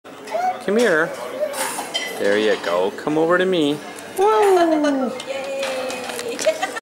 First Steps
Stevie's first consecutive (more than 3) steps on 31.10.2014.